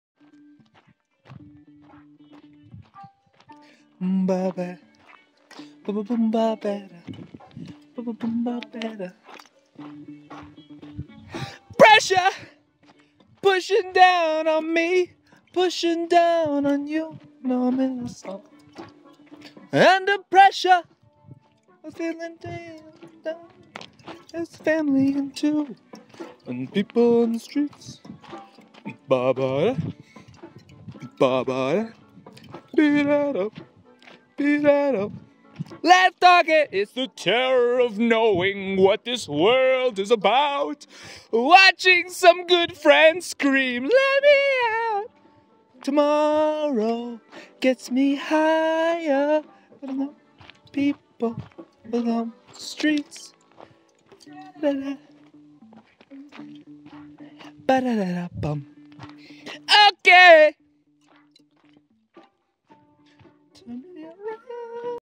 Vlog 32-04/25/2019
Just singing and walking around